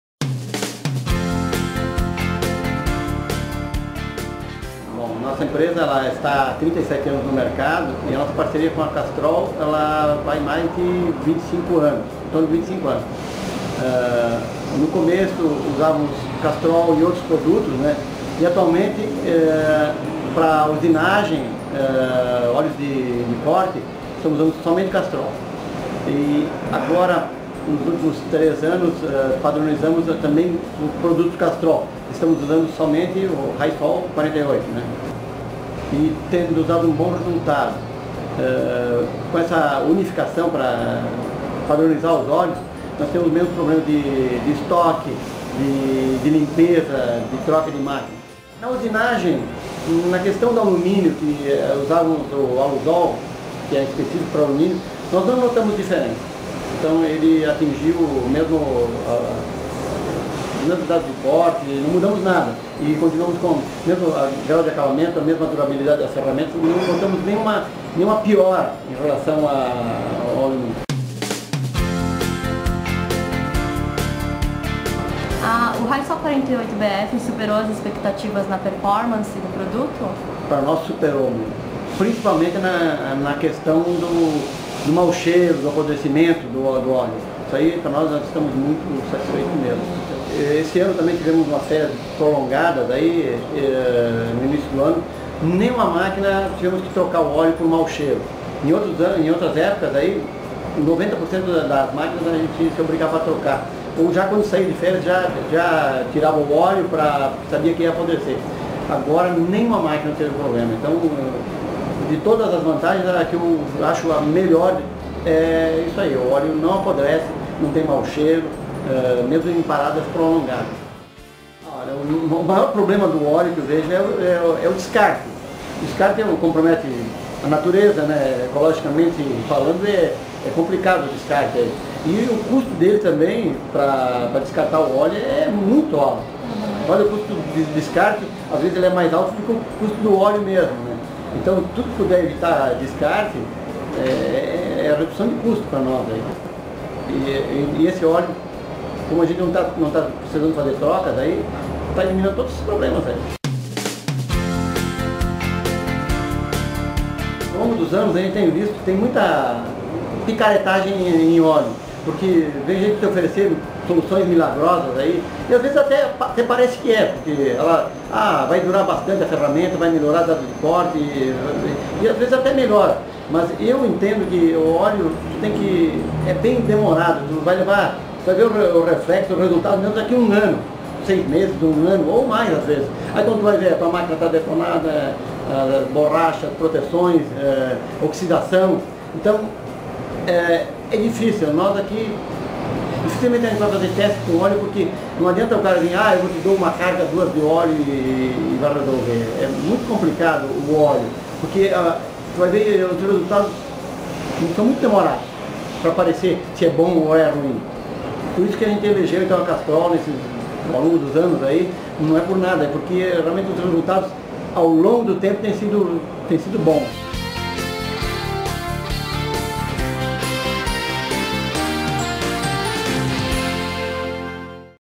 Castrol Entrevista: Rudimar Demore
O Diretor da Aços Peças Demore, Rudimar Demore, nos concedeu uma entrevista para falar sobre a parceria com a Castrol, que há  25 anos fornece serviços e produtos para sua empresa.